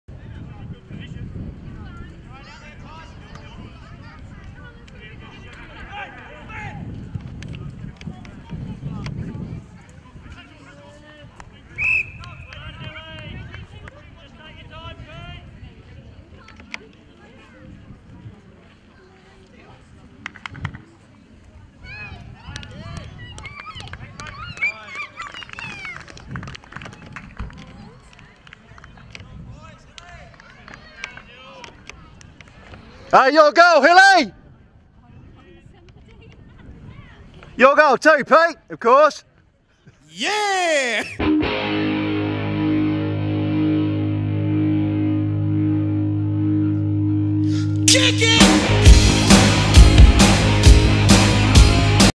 Old Dogs v South #2
From centre bounce to a goal in 52 seconds, including two replays.